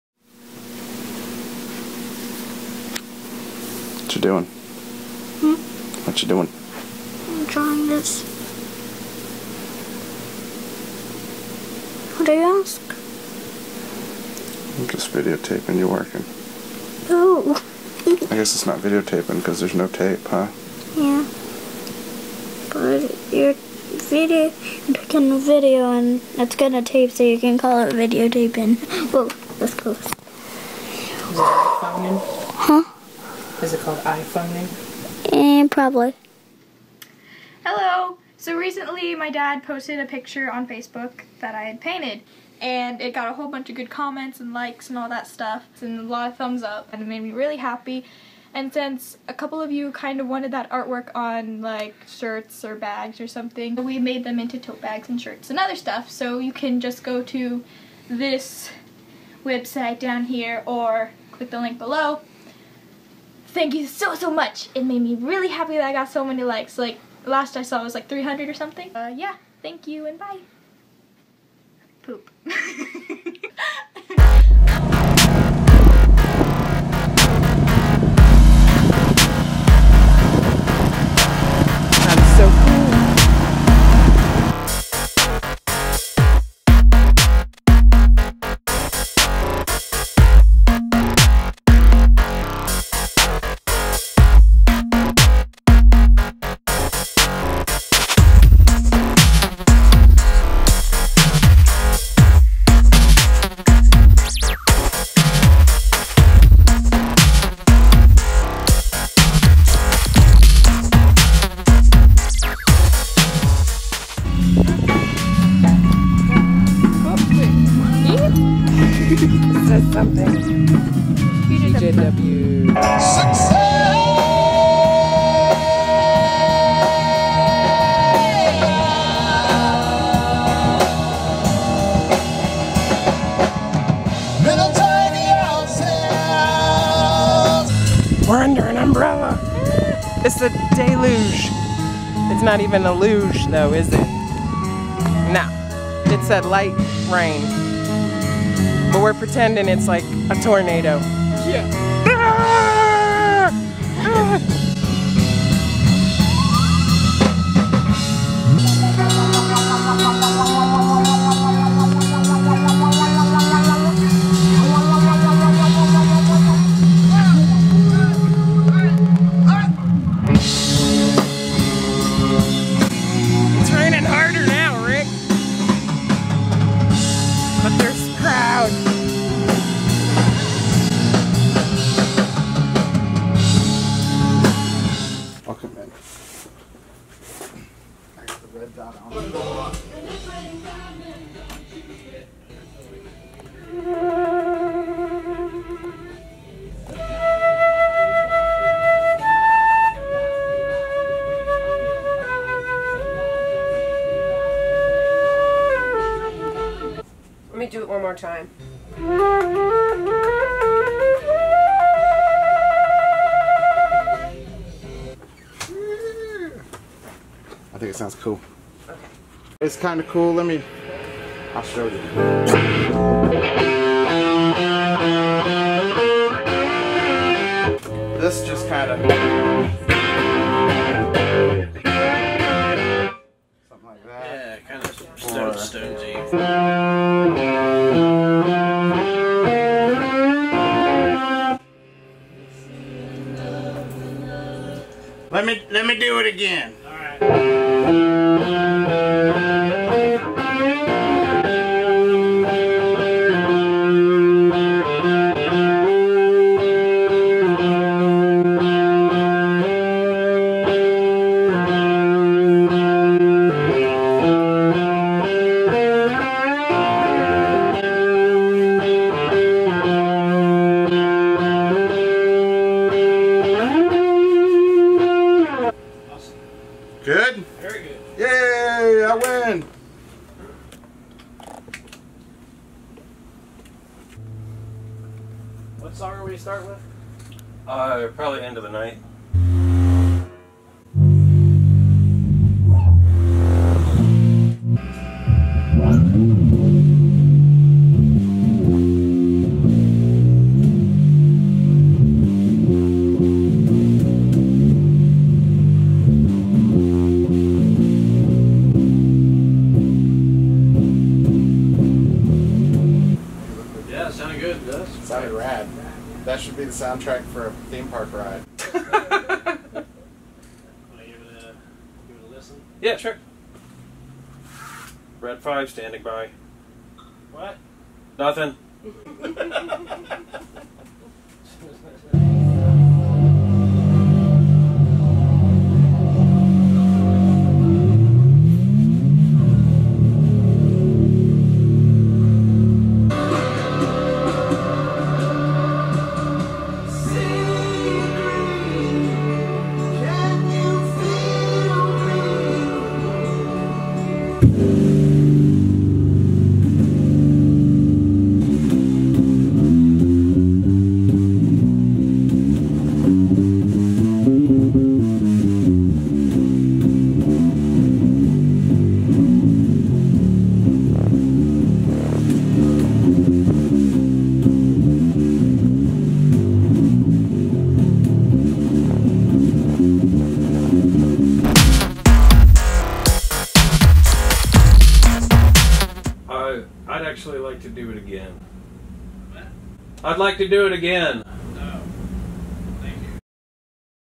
ART AND MUSIC | Episode 75 | THE WELLS FAMILY
What are the most important things in the world to the Wells Family? Beside each other, it's art and music. Checkout see of Presley's new artwork (available as art prints, shirts, totes and more at the link below) and get a peek at Cricketbows playing in a rain storm in a sculpture garden at the new Art Park in Vandalia and in the studio, finishing their latest recordings with Patrick Himes at Reel Love Recording Studio in Dayton, Ohio.